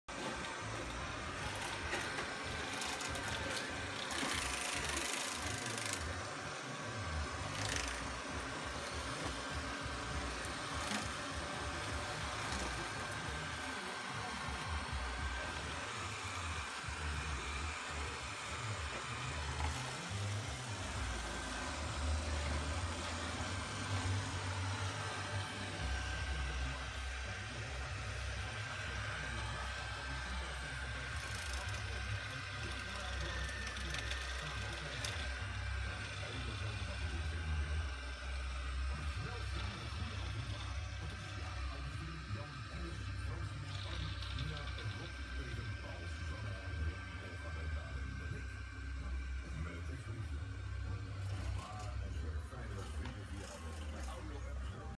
RoboRock S6 dweilen 2/4
Let op, week niet gedweild dus ietwat lastig schoon krijgen